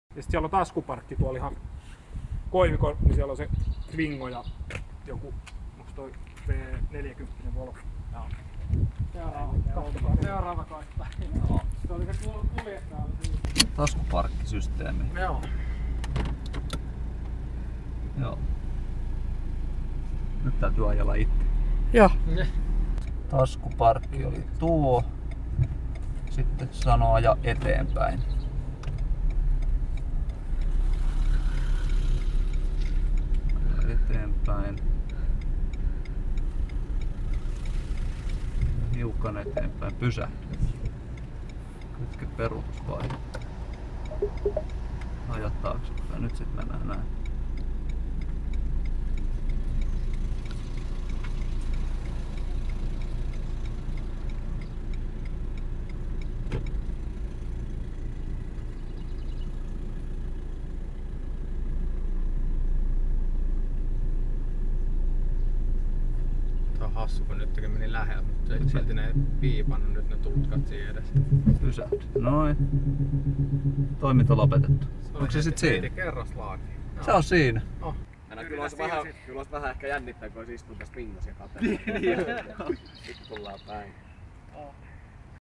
Renault Kadjarin automaattinen taskuparkkeeraus
Mekaanikoille ja työnjohtajille pidetyssä Renault Kadjarin uutuuskoulutuksessa opeteltiin, miten toimii auton itsensä tekemä taskuparkkeeraus.